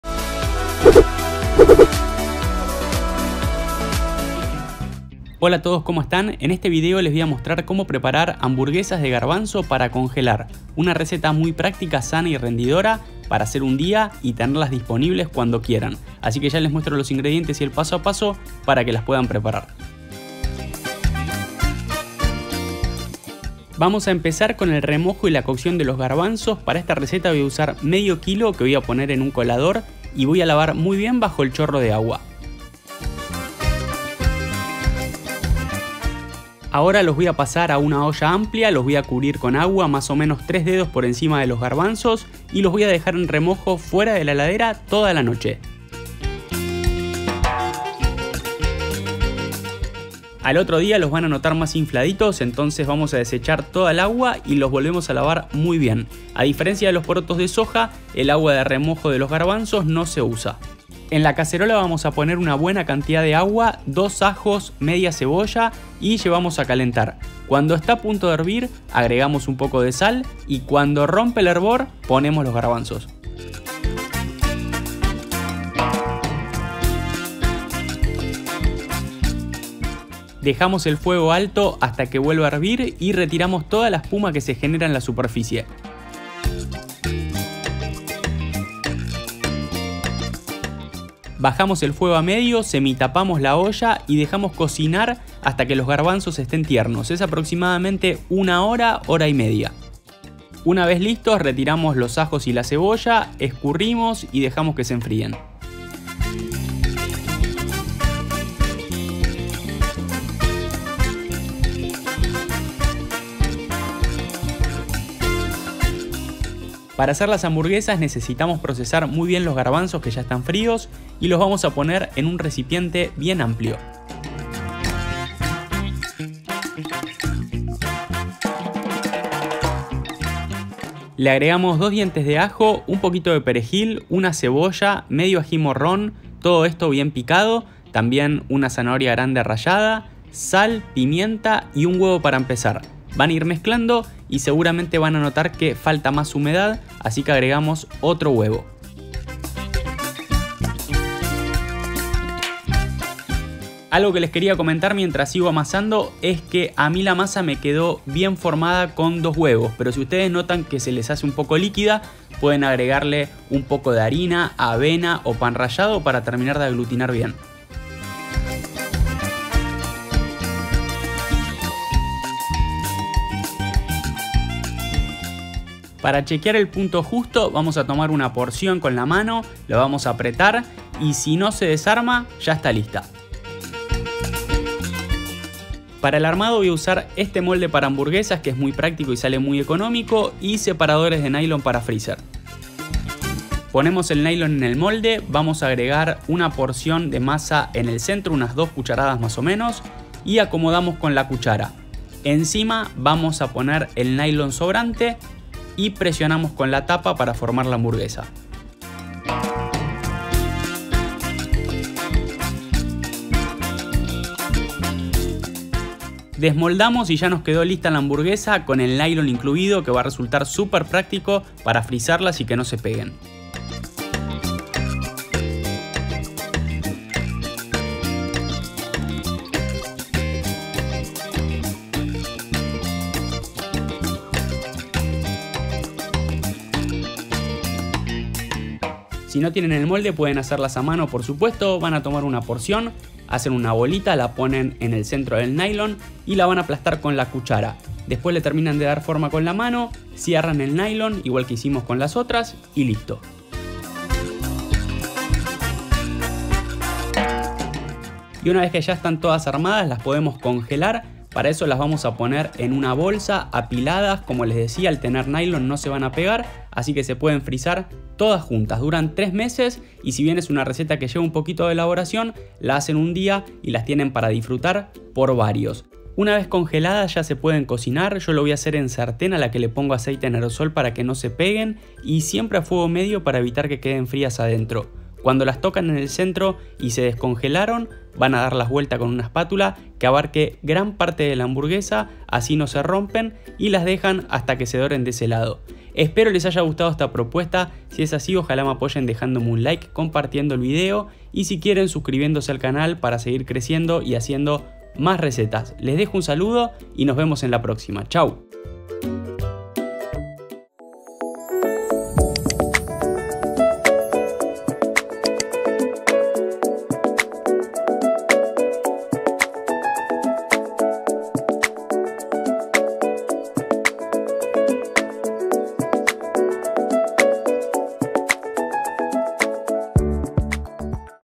Cómo Hacer HAMBURGUESAS DE GARBANZOS Caseras Para Congelar / MONO 1981
Cómo Hacer HAMBURGUESAS DE GARBANZOS Caseras Para Congelar

Hola! En este video les muestro como hacer hamburguesas de garbanzo caseras para congelar. Una opción muy práctica y rendidora para comer rico y sano.

Ingredientes (14 unidades aproximadamente)

Cocción de los garbanzos

500 gr de garbanzos secos
2 dientes de ajo
1/2 cebolla
Sal

Hamburguesas
2 dientes de ajo
1 manojo de perejil
1 cebolla grande
1/2 ají morrón grande
1 zanahoria grande
Sal
Pimienta
2 huevos

Importante

*Si necesitás podés agregar un poco de harina para aglutinar

*Si las querés más compactas podés pasarlas por huevo batido y empanizarlas (este procedimiento se hace con las hamburguesas congeladas)